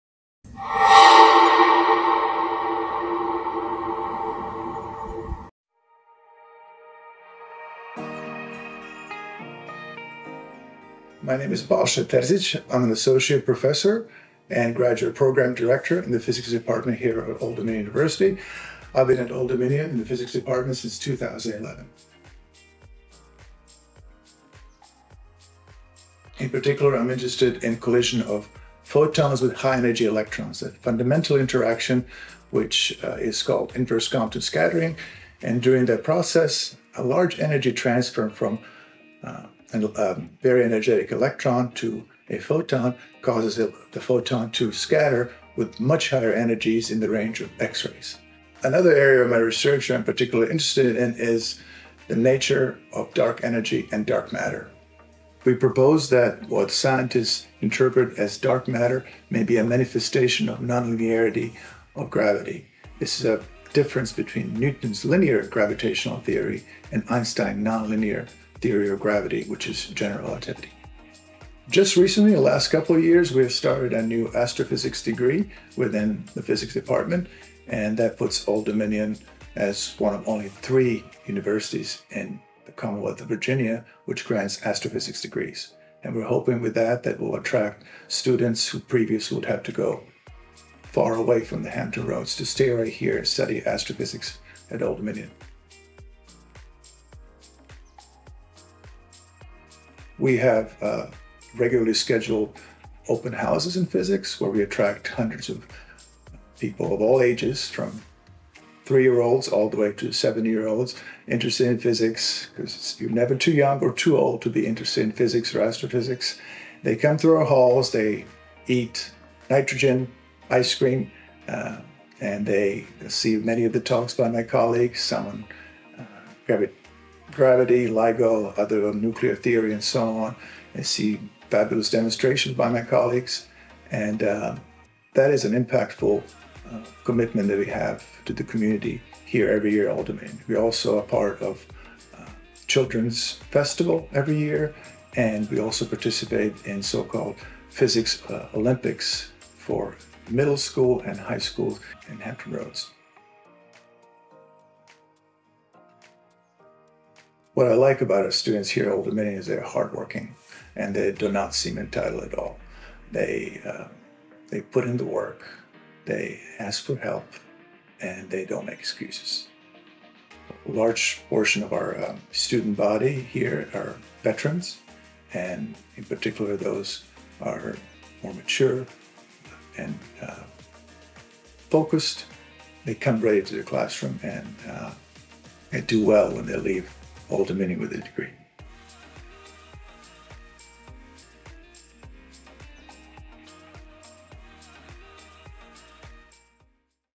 Meet ODU Physics Associate Professor Balša Terzić
Associate Professor Balša Terzić does research in the Department of Physics at Old Dominion University on astrophysics, accelerator physics, high-performance computation and nonlinear dynamics. He works with graduate students and works hard to share science to students of all ages in the Hampton Roads region and beyond.